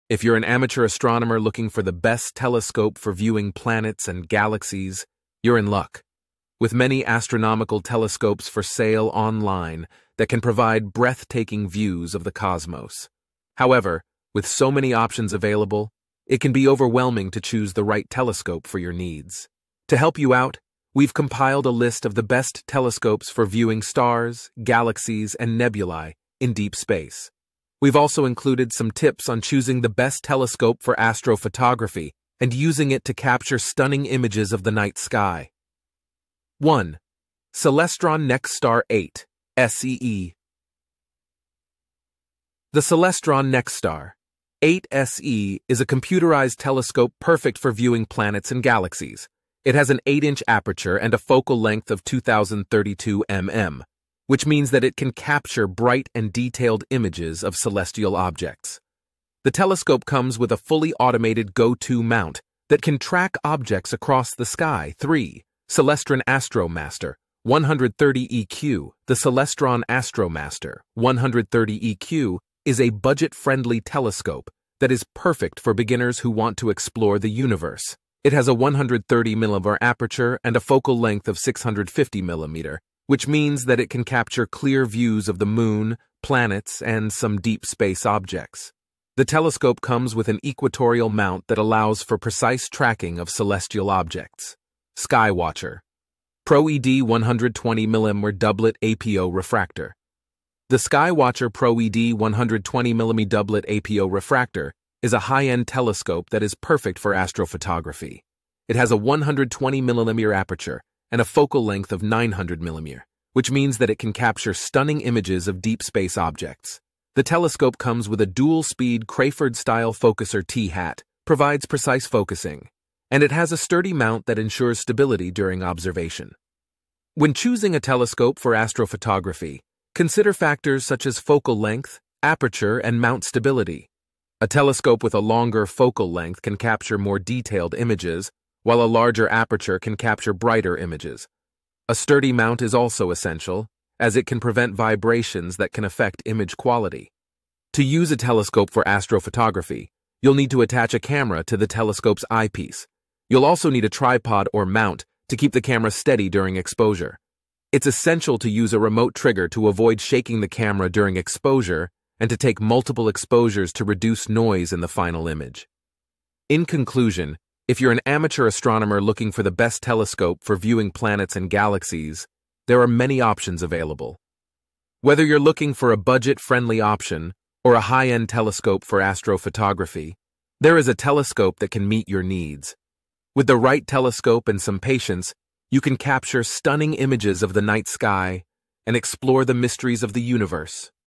Best Telescope for Viewing Deep Space
A top telescope for a beginner astronomer strikes a balance between ease of use, affordability, and the ability to provide satisfying views of celestial objects.

The ideal telescope for a beginner astronomer is a versatile and user-friendly instrument. It is designed to ignite a lifelong passion for stargazing. It’s a telescope carefully selected to ensure that those new to the hobby can effortlessly embark on their celestial journey with confidence and excitement.